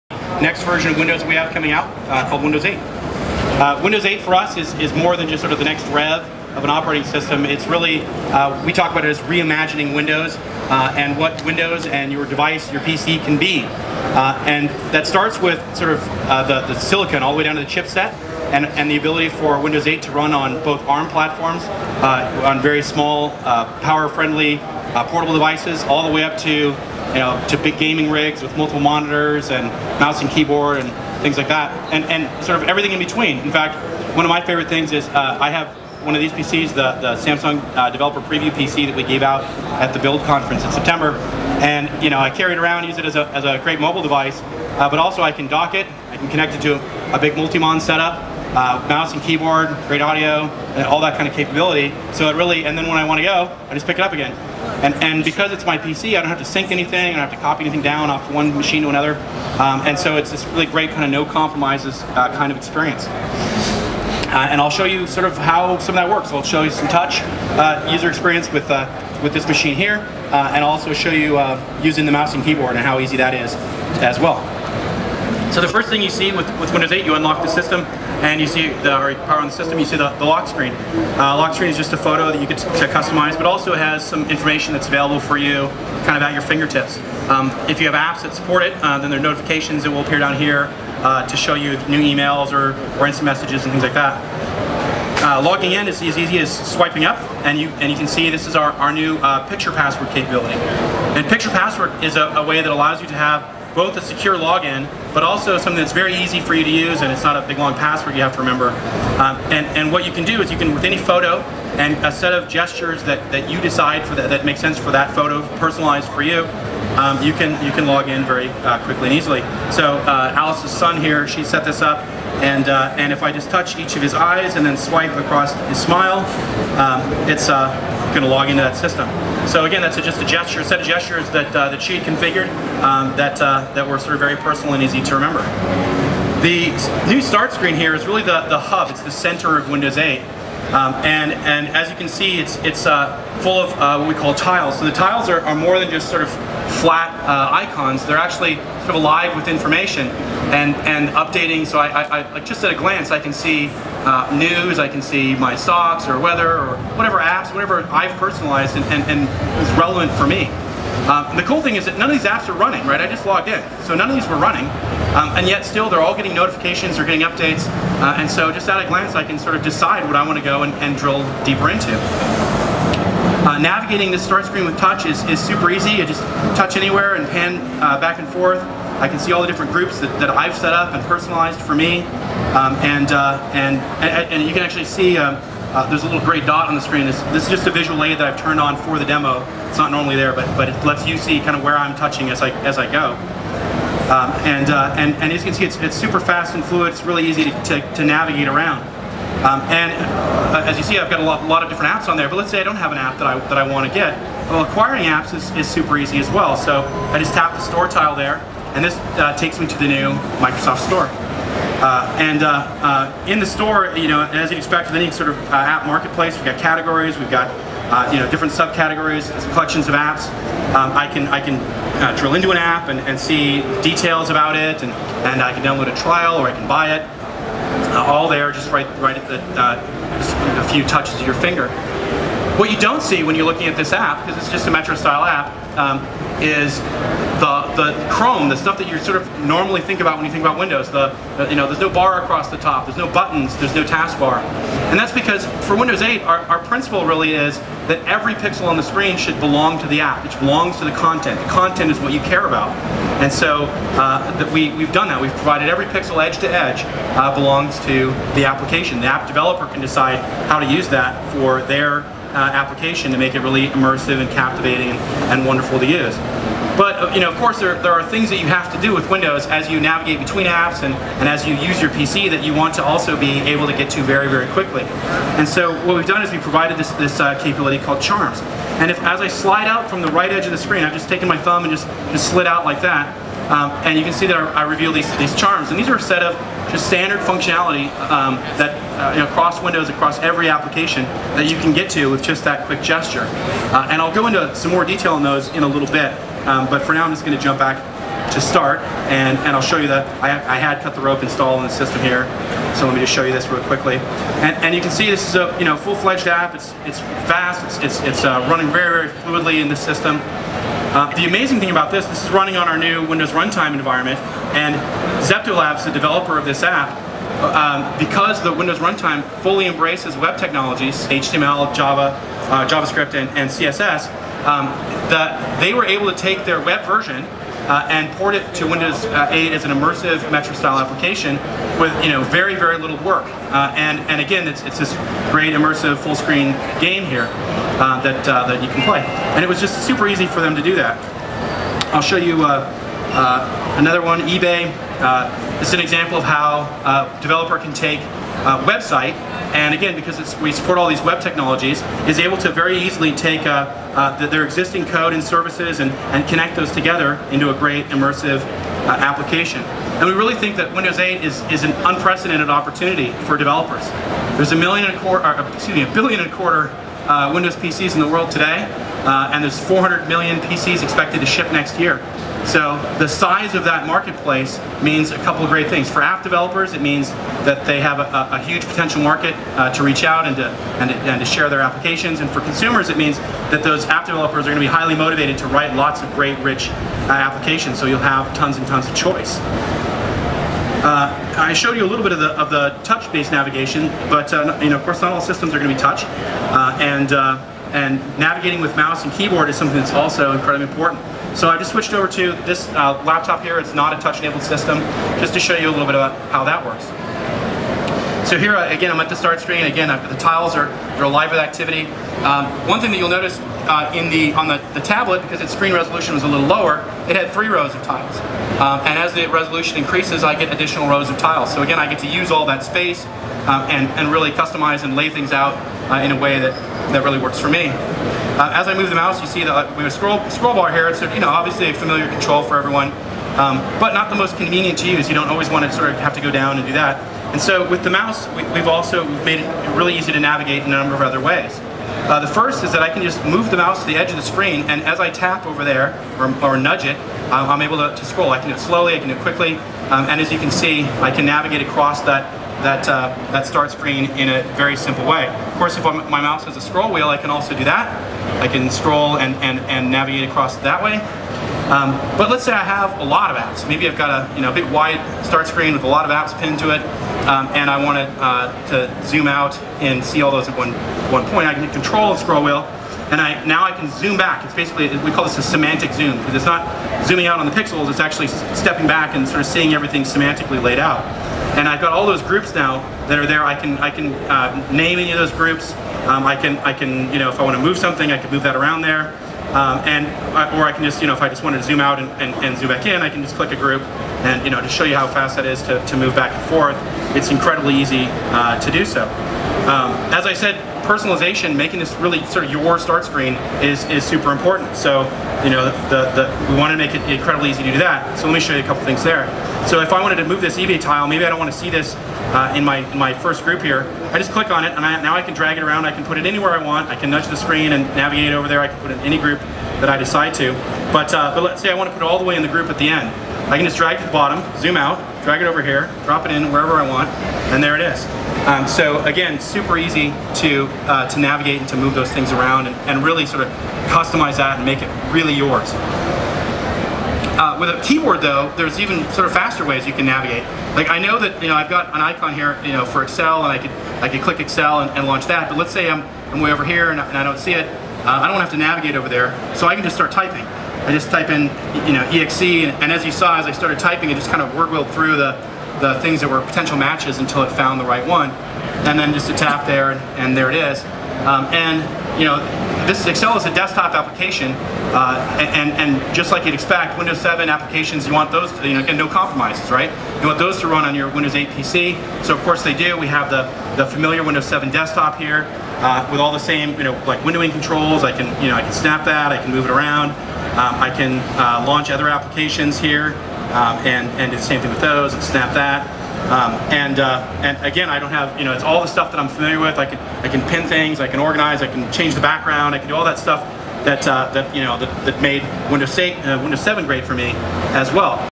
CES 2012 - Windows 8 Demo (Part 1 of 2)
We get a sneak peak at the next generation of Windows (part 1 of 2).